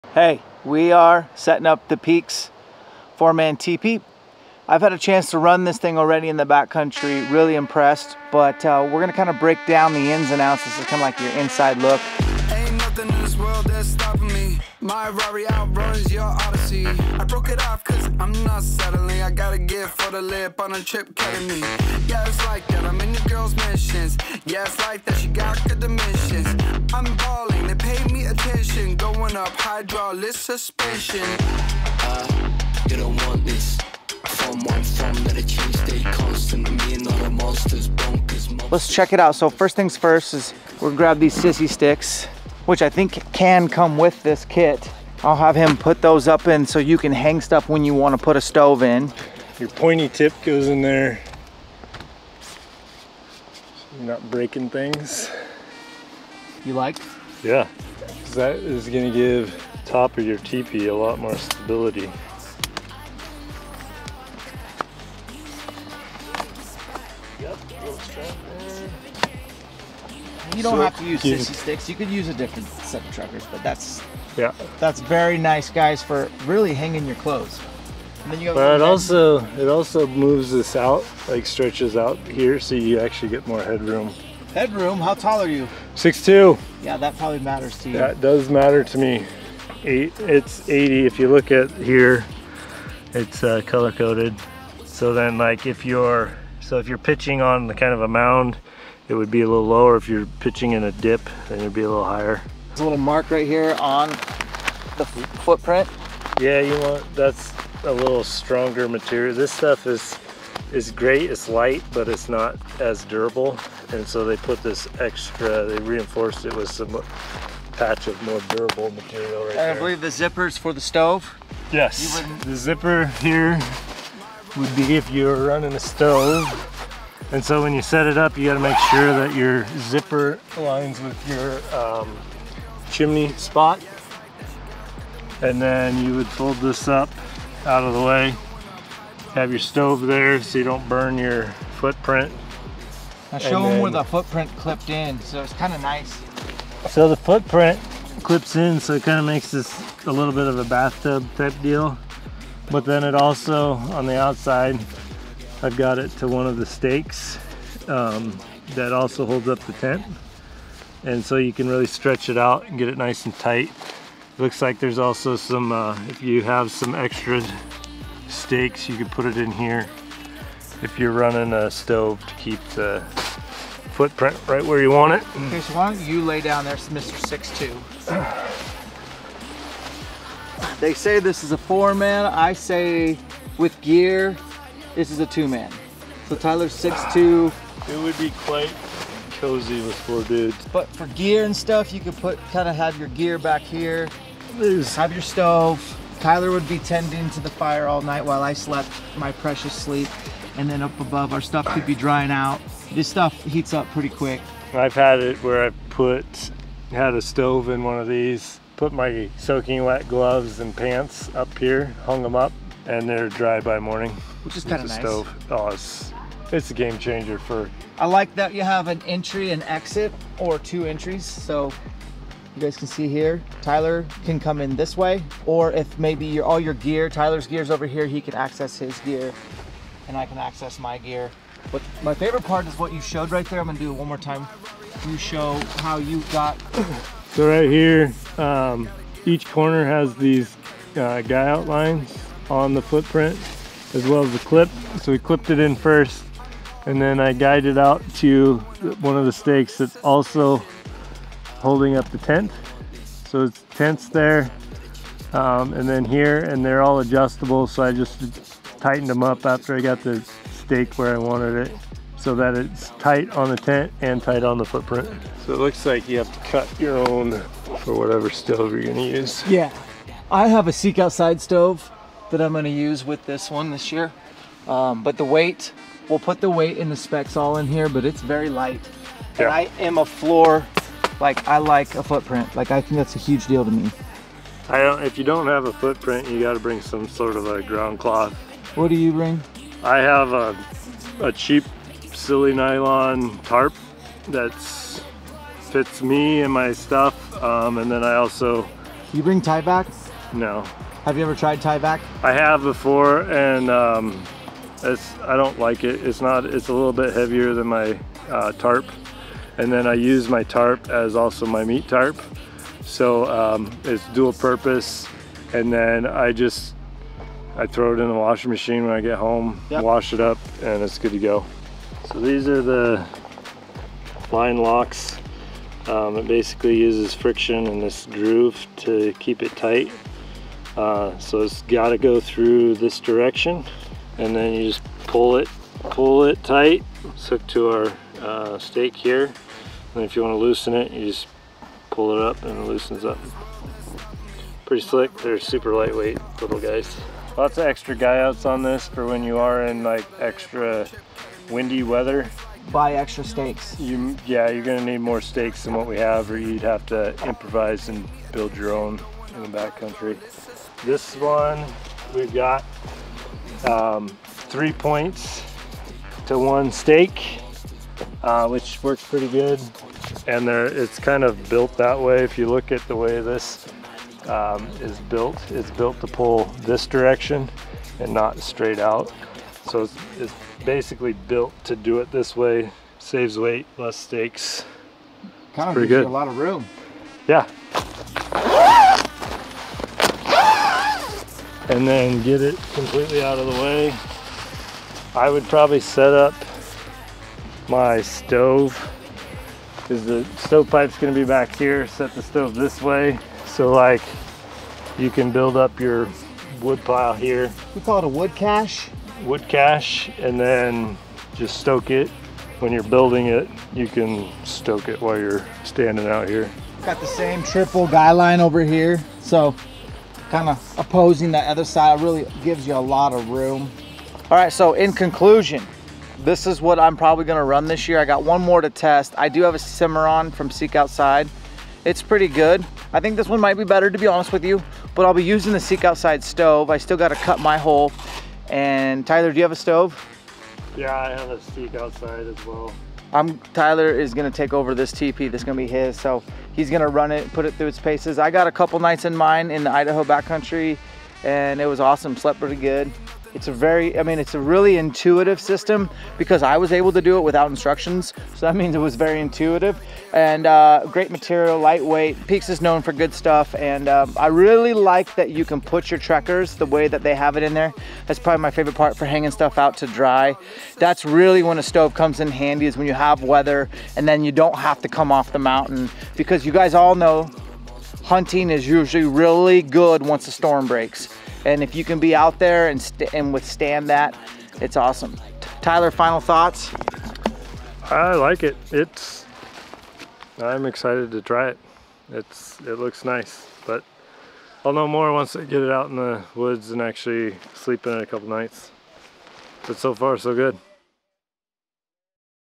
PEAX Solitude 4 Tipi Setup with ElkShape
Dan and the ElkShape crew set up the PEAX Solitude 4 Tipi and give a walk through of the tent. If you're new to camping with a Tipi give this a watch and see how easy they are to set up.